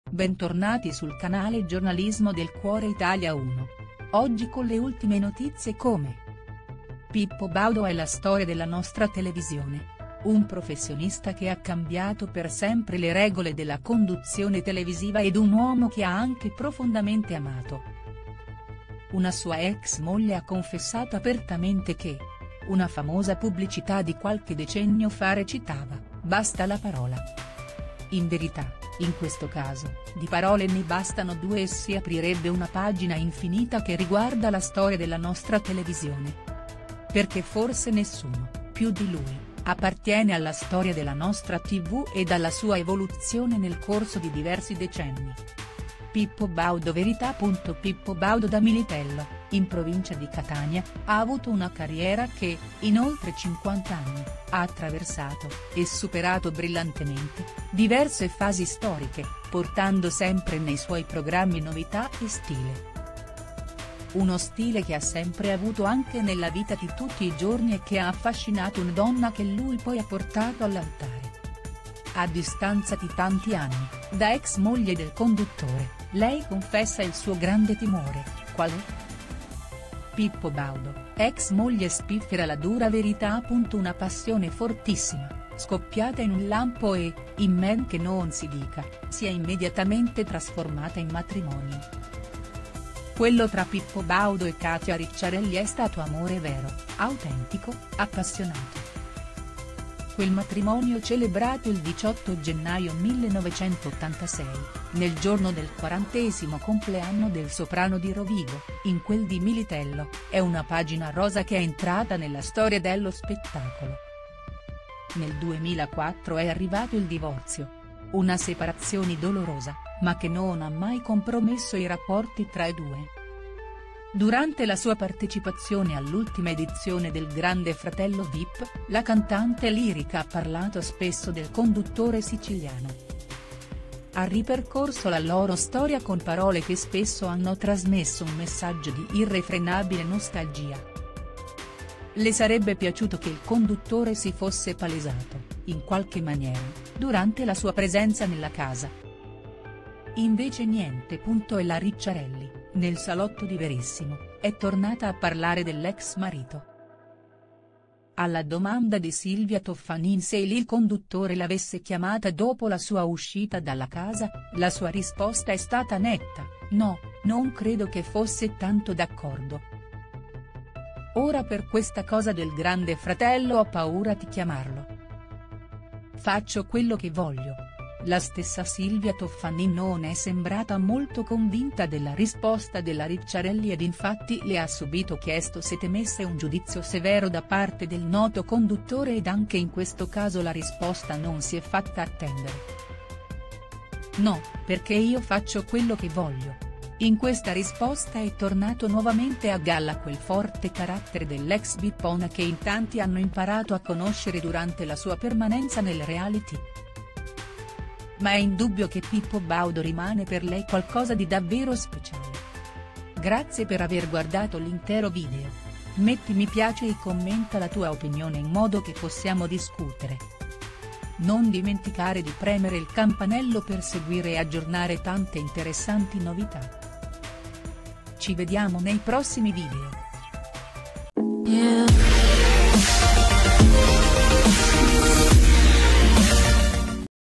Pippo Baudo, ex moglie spiffera la dura verità “ho paura a…”
Pippo Baudo è la storia della nostra televisione. Un professionista che ha cambiato per sempre le regole della conduzione televisiva ed un uomo che ha anche profondamente amato. Una sua ex moglie ha confessato apertamente che…

Una famosa pubblicità di qualche decennio fa recitava: Basta la parola. In verità, in questo caso, di parole ne bastano due e si aprirebbe una pagina infinita che riguarda la storia della nostra televisione. Perché forse nessuno, più di lui, appartiene alla storia della nostra tv ed alla sua evoluzione nel corso di diversi decenni.